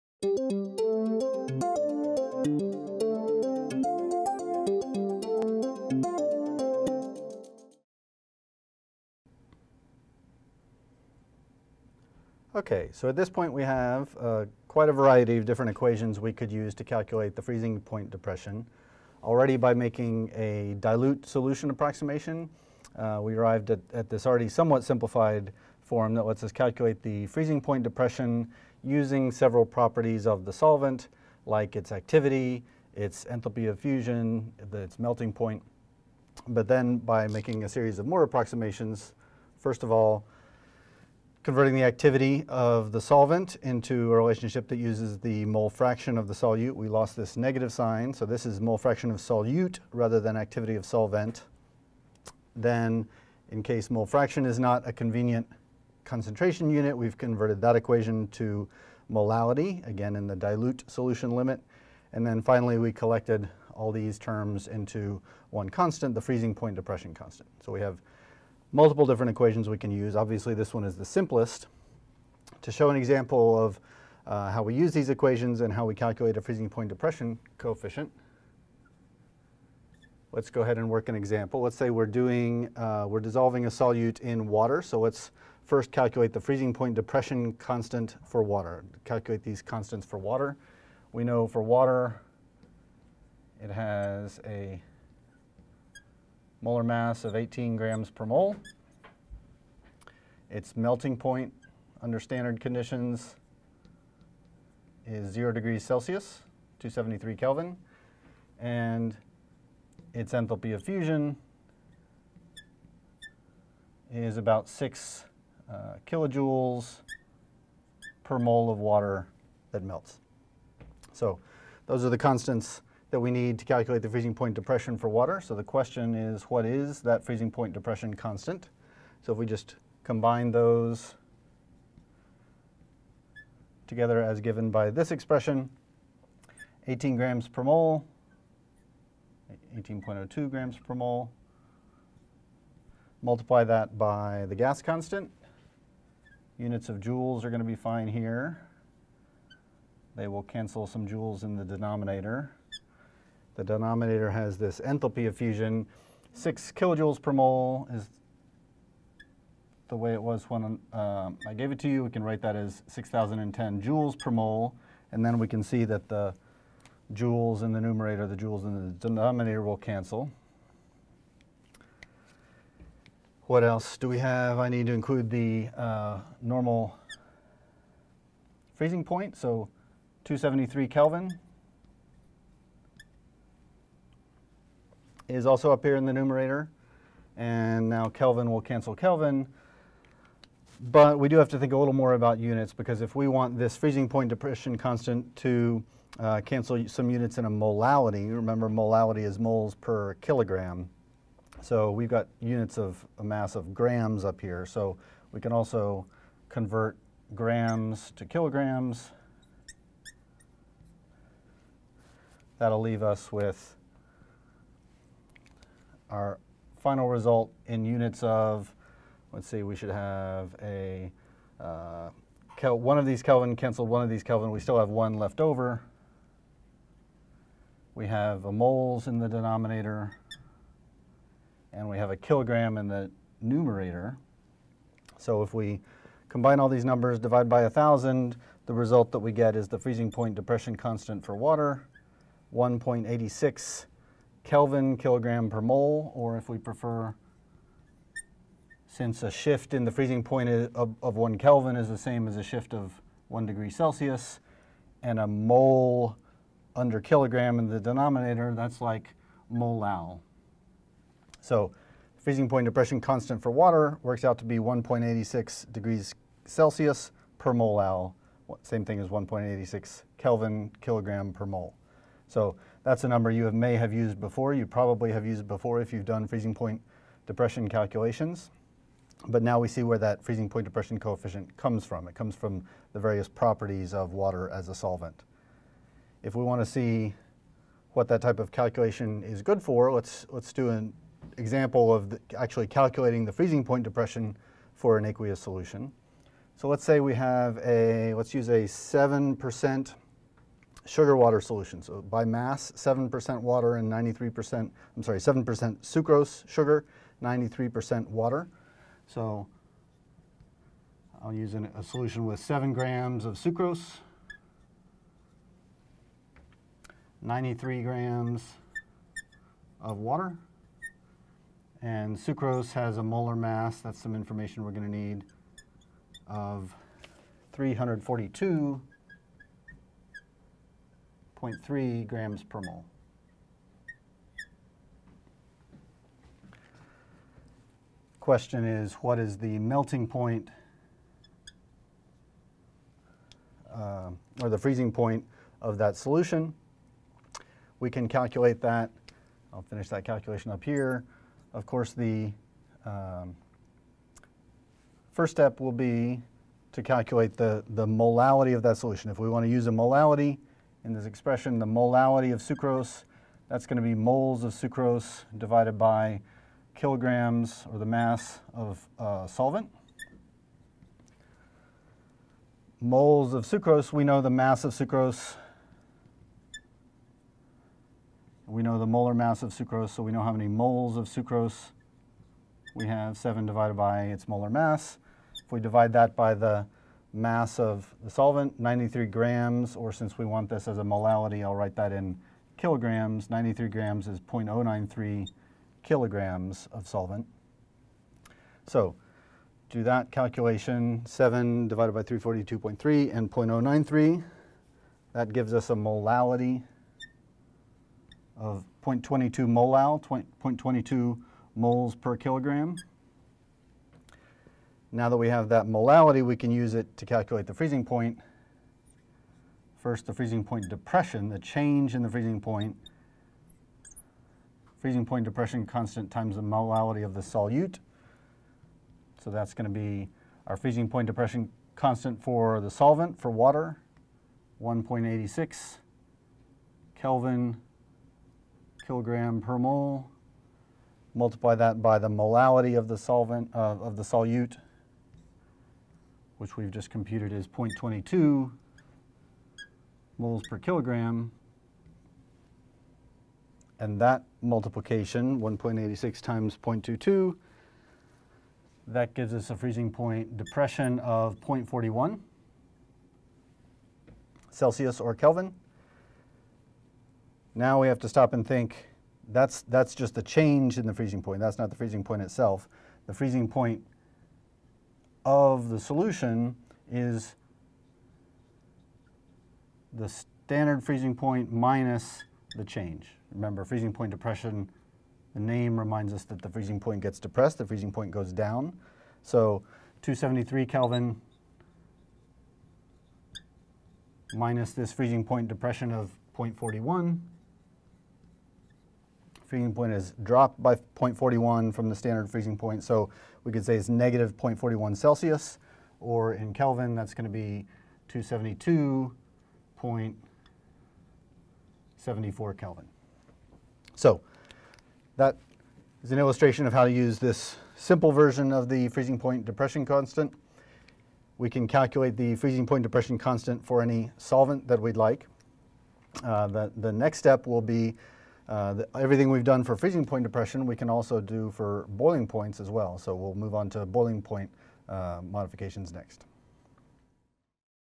Freezing Point Depression (Example)
The freezing point depression constant for a solvent can be calculated directly from its molar mass, freezing point, and enthalpy of fusion.

Using this freezing point depression constant, the freezing point for a solution in that solvent can be predicted.